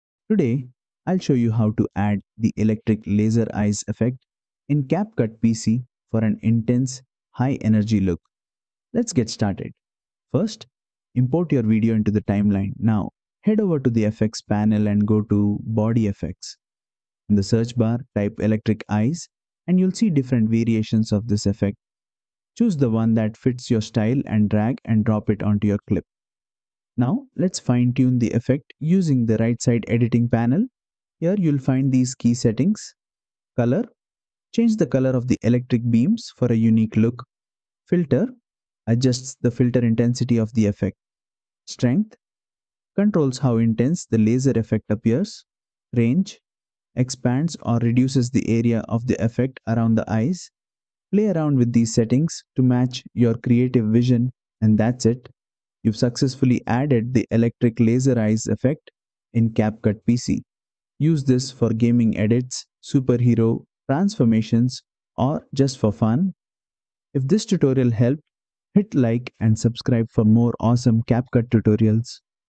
How to Add Electric Laser Eyes Effect in CapCut PC Desktop - Lesson 139 CapCut Tutorial
Want to give your video a supercharged, electric look? Add the Electric Laser Eyes effect in CapCut PC! This effect adds glowing, electric beams to the eyes, making your clips look powerful and intense.

Steps to Apply the Effect:

Import your video into the timeline.
Open the Effects panel and go to Body Effects.
Search for Electric Eyes and select the effect.
Drag and drop it onto your video.
Adjust the Color, Filter, Strength, and Range sliders to customize the look.